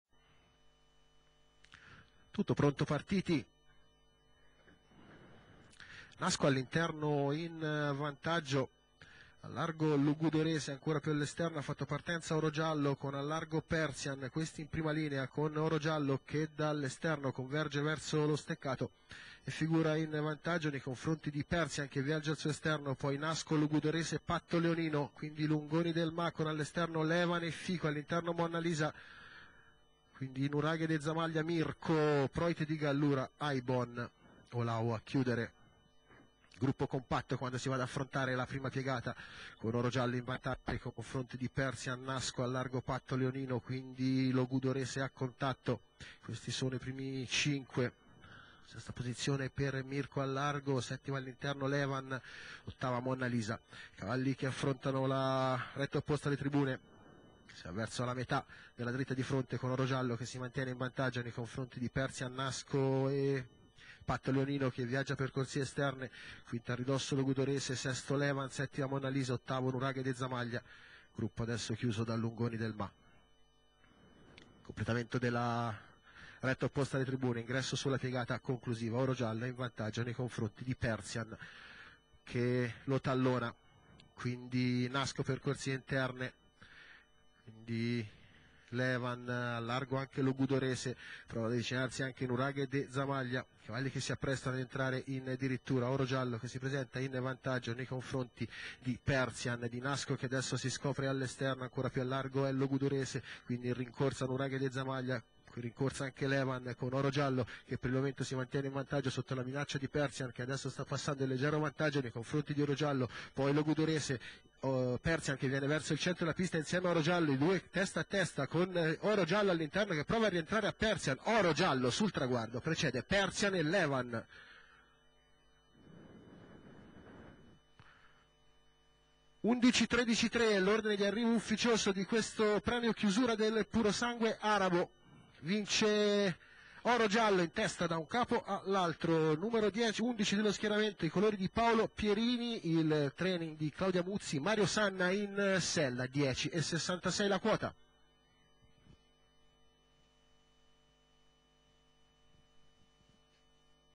Premio Chiusura del P.S.A 2012 (CONDIZIONATA)
31/10/2012 Ippodromo del Casalone di Grosseto, scendono in pista i cavalli di purosangue arabo di 3 anni ed oltre, sui 1.750 della pista grande..A conquistare la vittoria sono OROGIALLO e Mario Sanna, che battono PERSIAN e Mauro Manueddu..terza moneta che va a LEVAN e Marco Biagiotti..